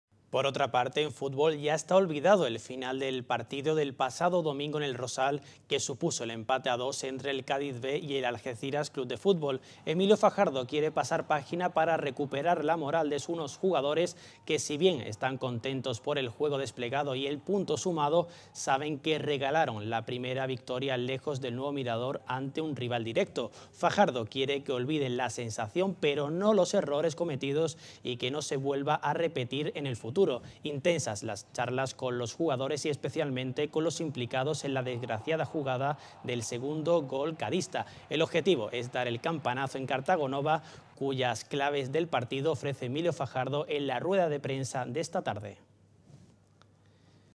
El Algeciras quiere rematar la faena el domingo con una victoria
Por otra parte, en fútbol, ya está olvidado el final del partido del pasado domingo en el Rosal que supuso el empate a dos entre Cadiz B y Algeciras CF. Emilio Fajardo quiere pasar página para recuperar la moral de unos jugadores que , si bien están contentos por el juego desplegado y el punto sumado, saben que regalaron la primera victoria lejos del Nuevo Mirador a un rival directo. Fajardo quieren que olviden la sensación pero no los errores cometidos y que no se vuelvan a repetir en el futuro. Intensas las charlas con los jugadores y especialmente con los implicados en la desgraciada jugada del segundo gol cadista. Objetivo dar el campanazo en Cartagonova, cuyas claves del `partido ofrece Emilio Fajardo en la rueda de prensa de esta tarde.